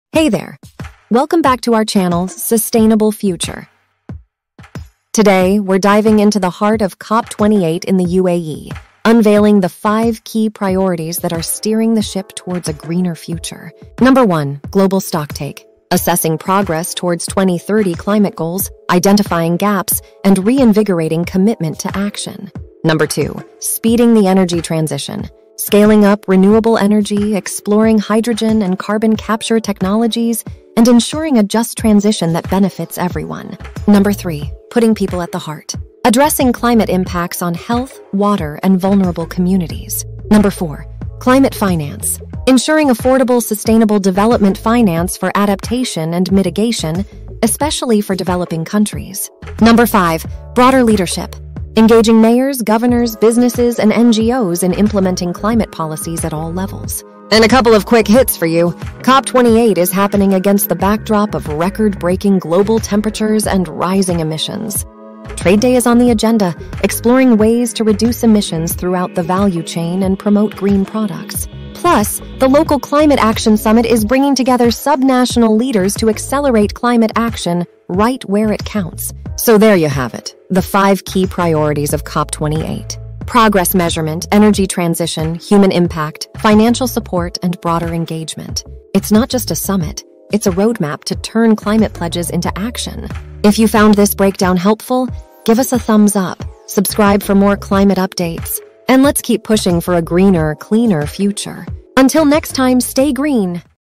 5 COP28 Key Priorities
The latest data on global climate urgency is an urgent call to action ahead of COP28, as the National Oceanic and Atmospheric Administration reports the warmest September on record. With the highest surface temperatures recorded throughout 2023, COP28, organized around themes like Technology and Innovation, Inclusion, Frontline Communities, and Finance, faces the challenge of addressing the escalating climate crisis. With COP28 approaching the halfway point for meeting climate goals by 2030, the summit will conduct a "global stocktake" to assess progress and reignite commitments. The summit will address crucial issues, including speeding the energy transition, prioritizing people in climate action, addressing climate finance and global trade impacts, and engaging broader leadership for effective climate change mitigation. Climate and environmental risks loom large in global perception, emphasizing the imperative for swift and inclusive action.

source - world economic forum